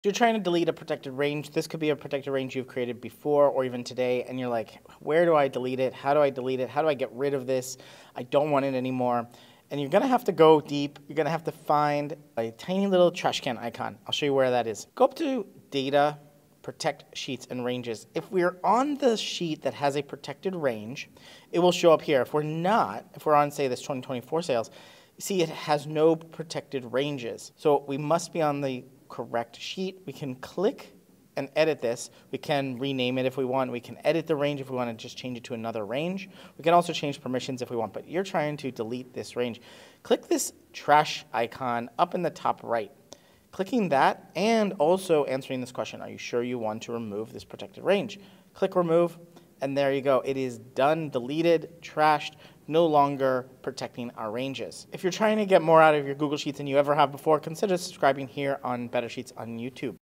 How to Delete Protected Range in Google Sheets
How to Delete a Protected Range in Google Sheets

In this tutorial, we walk you through the steps to delete a protected range in Google Sheets. Learn how to navigate to the 'Protect sheets and ranges' menu, identify the correct sheet with the protected range, and use the trash icon to remove the protection. Perfect for anyone looking to delve deeper into Google Sheets functionalities.

00:00 Introduction to Deleting Protected Ranges
00:14 Navigating to the Protected Range
00:29 Editing and Deleting the Protected Range
00:39 Final Steps and Confirmation
00:54 Conclusion and Subscription Prompt